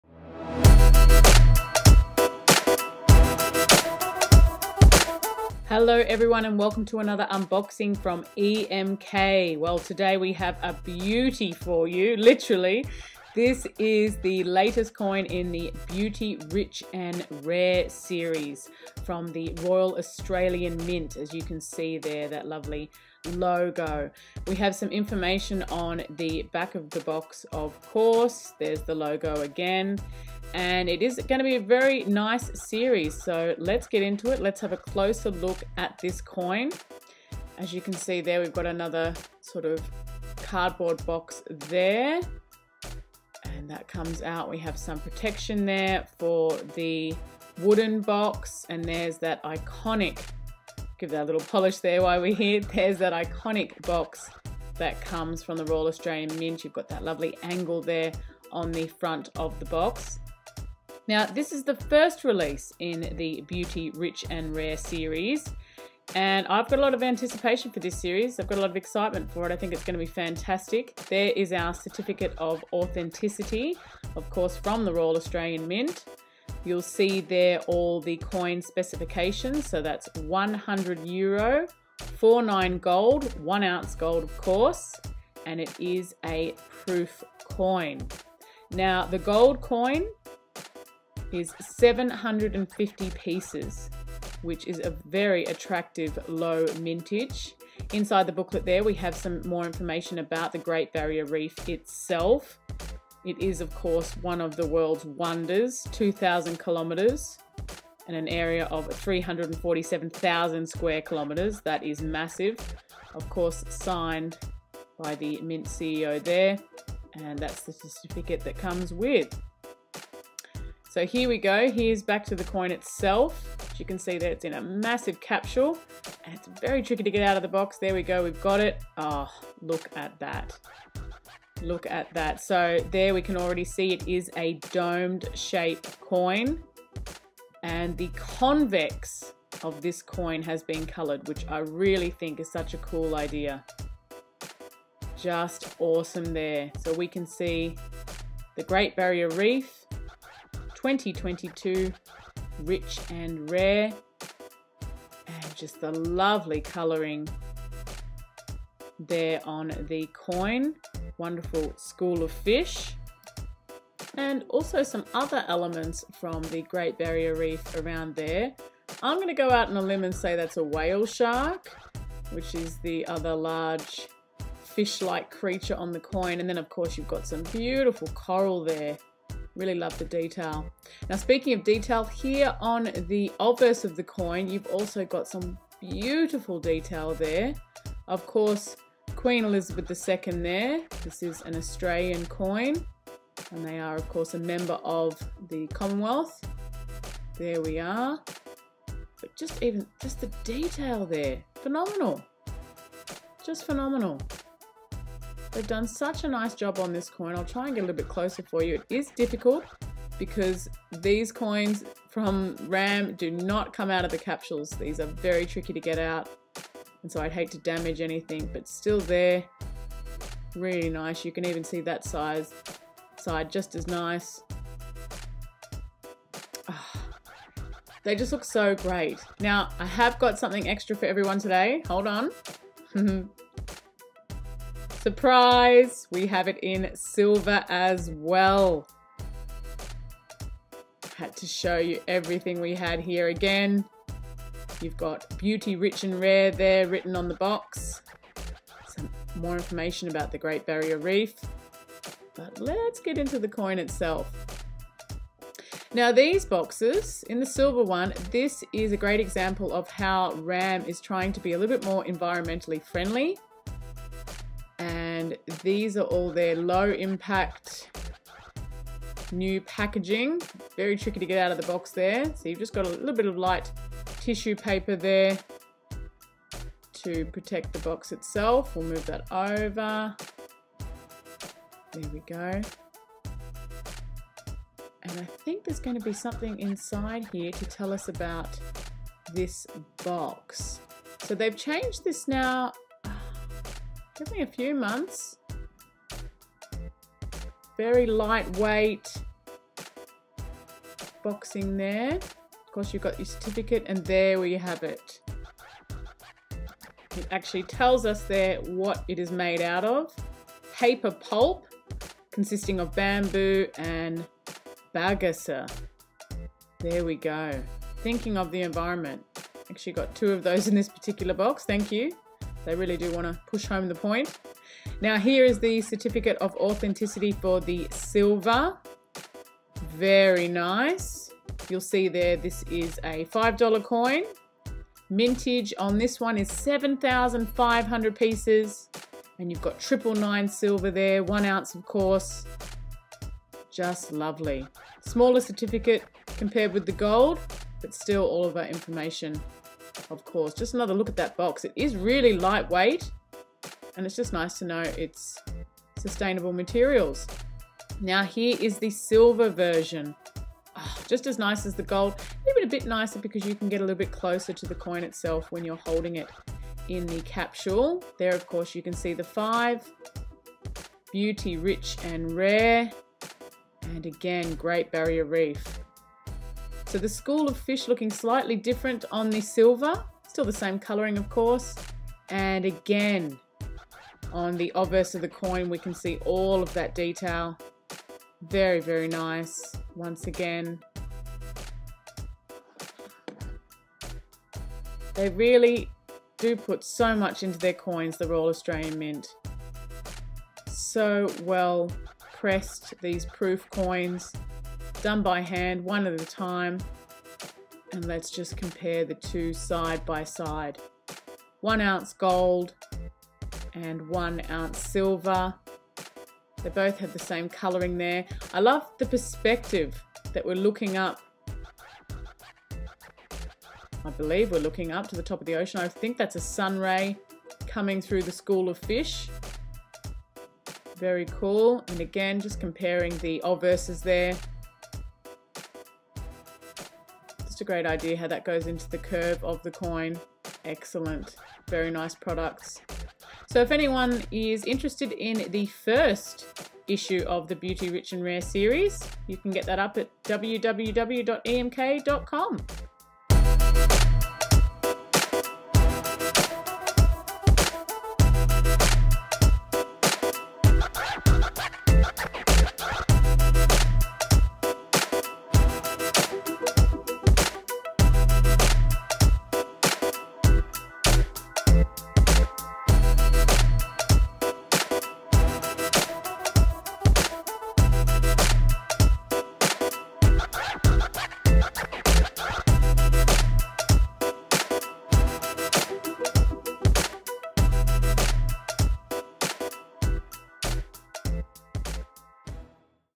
COIN OF THE MONTH ! Beauty, Rich & Rare Great Barrier Reef 1 Oz Curved Silver & Gold Coin in Color
The first issue of the new Series Beauty, Rich & Rare was struck by the Royal Australian Mint from one ounce of 99.9% silver in proof quality and is dedicated to the Great Barrier Reef. The convex side of the domed coin features a partially colored depiction of the maritime natural wonder. A colorful school of fish stretching across the motive is surrounded by other inhabitants of the coral reef. The inscriptions "GREAT BARRIER REEF", "2022 BEAUTY, RICH & RARE" and the denomination "$5" complete the design. The domed shape of the silver coin adds depth to the design, creating a particularly realistic effect. The reverse shows another depiction of the reef, the portrait of Queen Elizabeth II as well as the weight and the inscription "AUSTRALIA".

The first issue of the new Series Beauty, Rich & Rare was struck by the Royal Australian Mint from one ounce of 99.99% pure gold in proof quality and is dedicated to the Great Barrier Reef. The convex side of the domed coin features a partially colored depiction of the maritime natural wonder. A colorful school of fish stretching across the motive is surrounded by other inhabitants of the coral reef. The inscriptions "GREAT BARRIER REEF", "2022 BEAUTY, RICH & RARE" and the denomination "$100" complete the design. The domed shape of the Gold coin adds depth to the design, creating a particularly realistic effect. The reverse shows another depiction of the reef, the portrait of Queen Elizabeth II as well as the weight, the fineness and the inscription "AUSTRALIA".